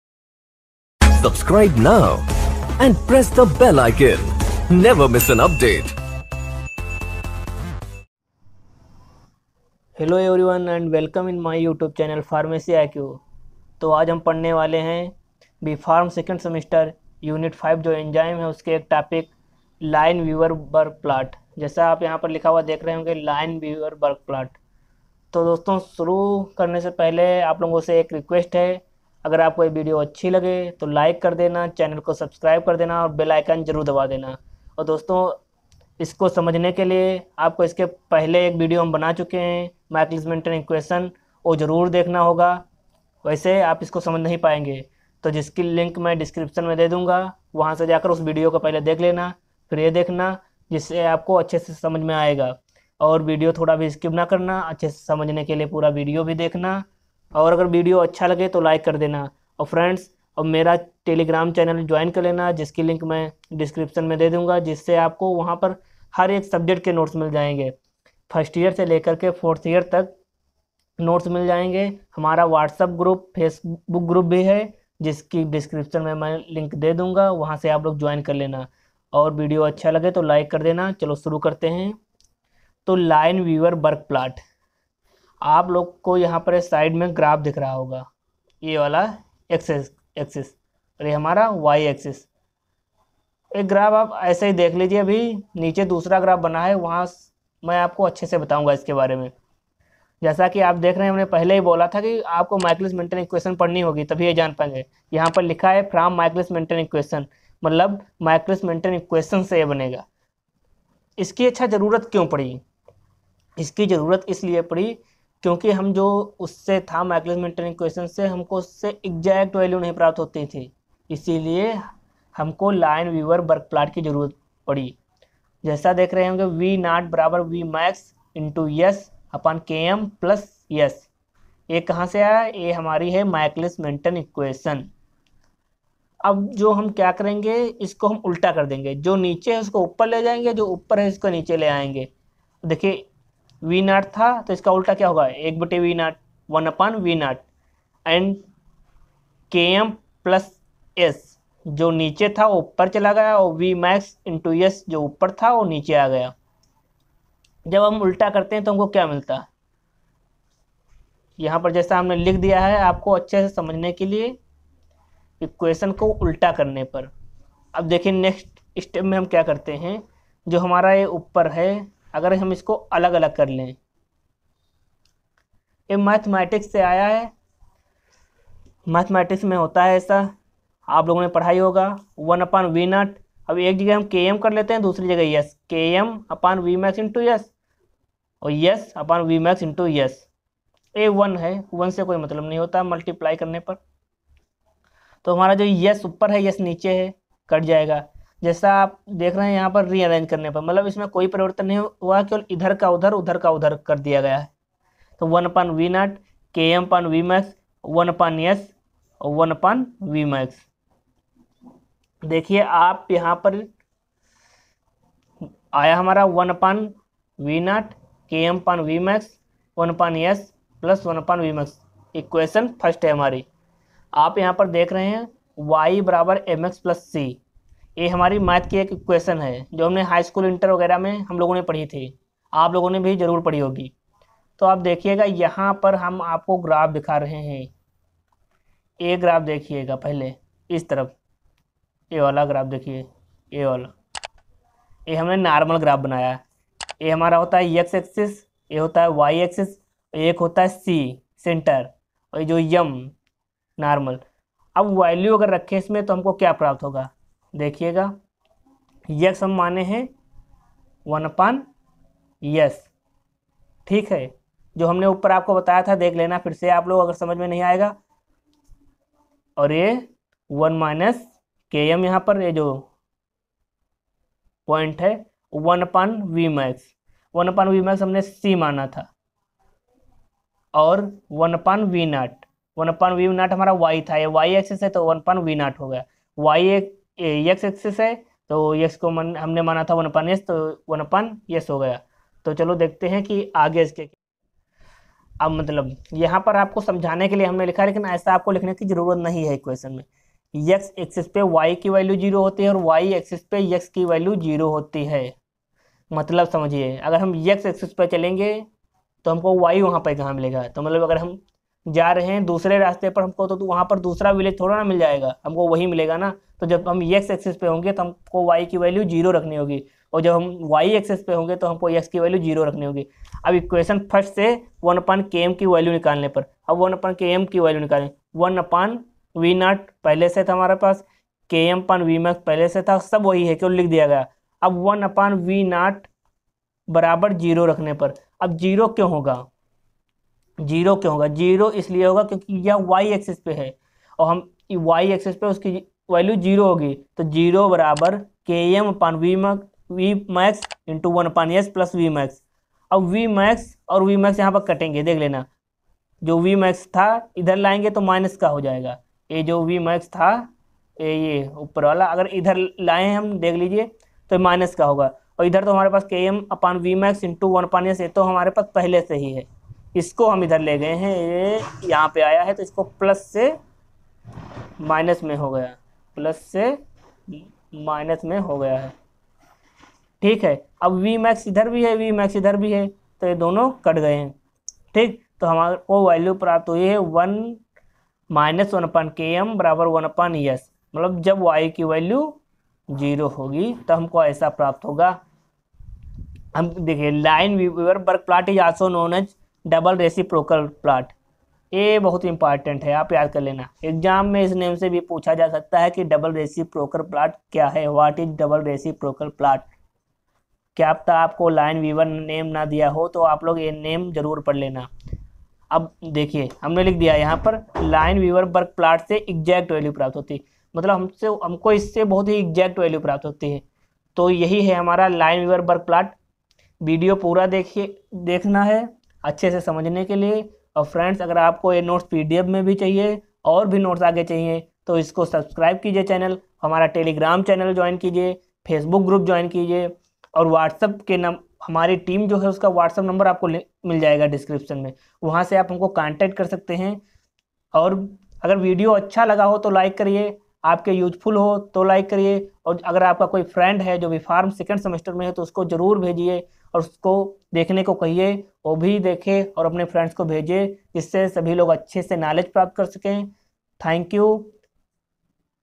Enzyme kinetics (Line Weaver Burke plot)|| Enzymes || Unit-5 || Biochemistry B.Pharmacy Syllabus PCI
This video Explains in Line weaver burke plot (In Hindi and English Language)

Topic Covered ✅
Enzyme Kinetics (Line weaver burke plot)
In very easy way
Unit-5  Enzyme

मैं भी एक student ही हूं अगर कोई गलती हो गई हो तो माफ कर देना।